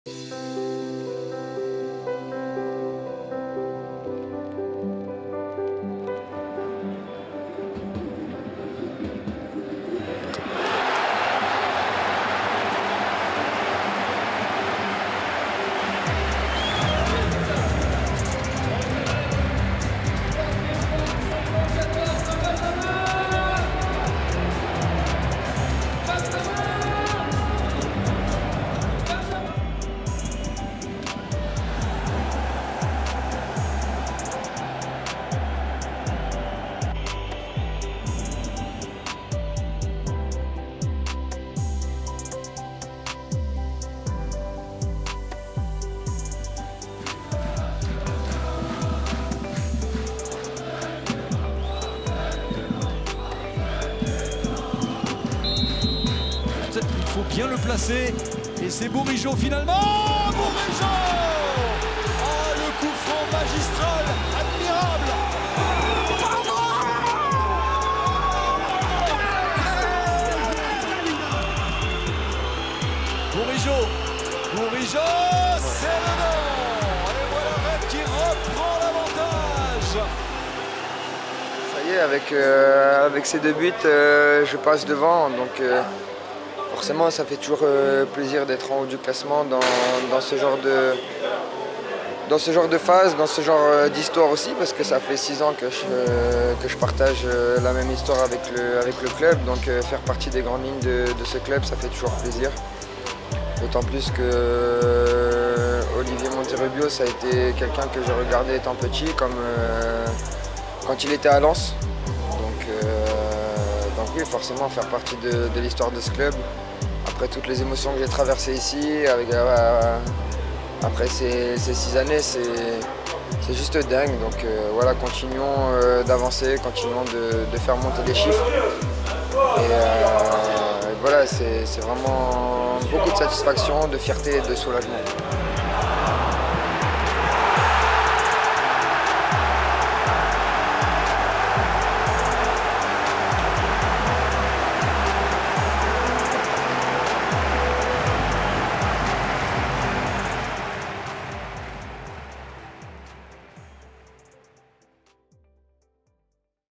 🥇 NEW RECORD | Benjamin Bourigeaud dépasse Olivier Monterrubio
50 buts - 54 passes décisives c'est le bilan affiché par Benjamin Bourigeaud depuis son arrivée au Stade Rennais F.C. en juin 2017. Double buteur à Brest, il bat le record d'Olivier Monterrubio décisif à 102 reprises lors de son passage à Rennes (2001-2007)